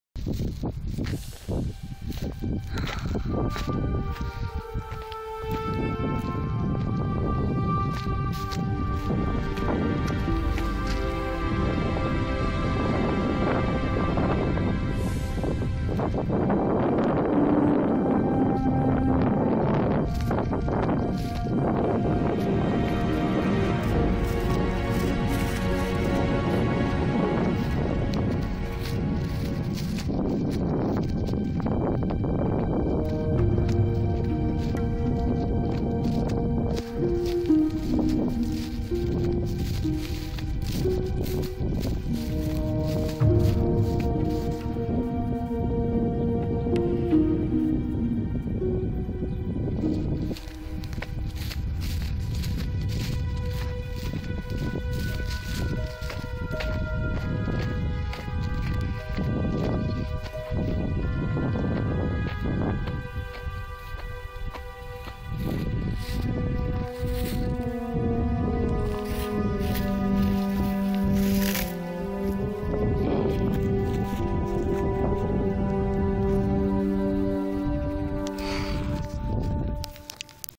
Real life Elwynn Forest (World Of Warcraft)
I took my mount for a ride , and after a while i ended up in elwynn where i had the chance to  gather some plants for my herbalism..=]